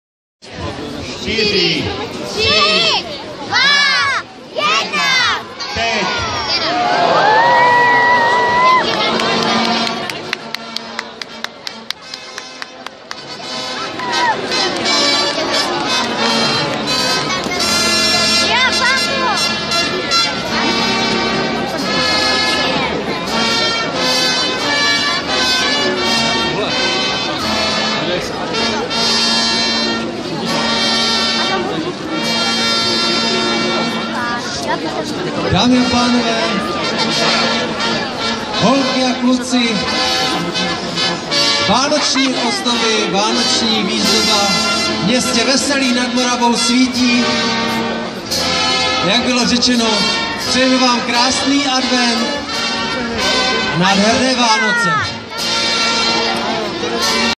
Veselí nad Moravou Vánoční strom
Rozsvicení Vánočního stromu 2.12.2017 Veselí nad Moravou